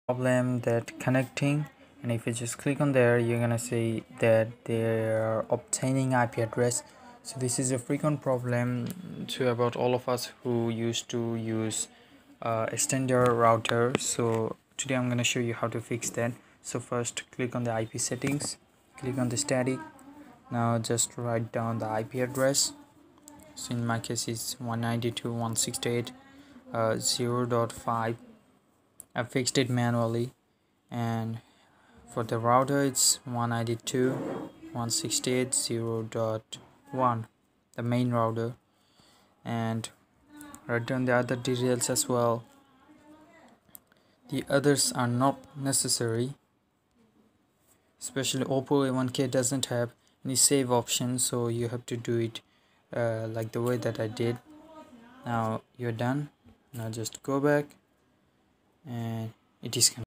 How To Fix Obtaining Ip Address problem - Wifi not connecting problem in Extender Mode
Check the part 2 of this video in my channel